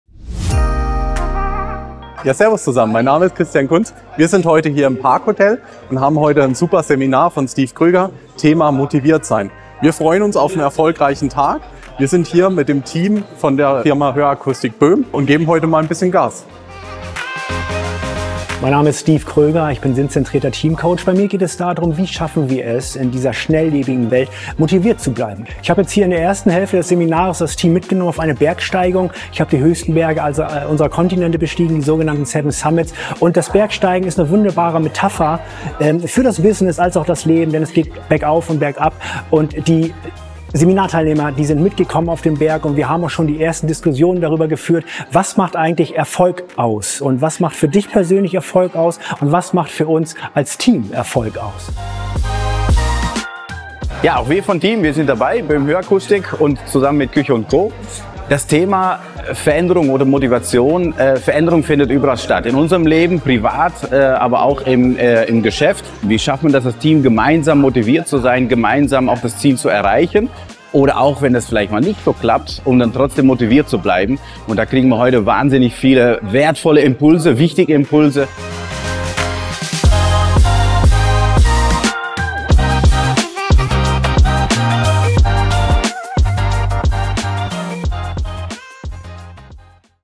Starke Teams in schwierigen Zeiten: Motivation und Resilienz fördern mit Steve Kröger
Erleben Sie mit uns eine inspirierende Reise zur Stärkung von Teamgeist und Resilienz! Gemeinsam mit BÖHM Hörakustik, Küche & Co haben wir in diesem Workshop viel über uns selbst gelernt.

In Zeiten von Veränderungen und Herausforderungen haben wir erfahren, wie wichtig es ist, bei uns selbst zu bleiben.
Wir haben gemeinsam neue Perspektiven und Ideen für die Zukunft entwickelt.

Ein besonderer Dank geht an Steve Kröger für diesen wundervollen Tag im Park Hotel Pforzheim.

Mit dem heutigen Input von Steve blicken wir voller Energie in die Zukunft!
ihr Fabian Böhm

BÖHM-Hörakustik:
Hier alle Links und Social Media Kanäle, auf denen wir mit Ihnen kommunizieren: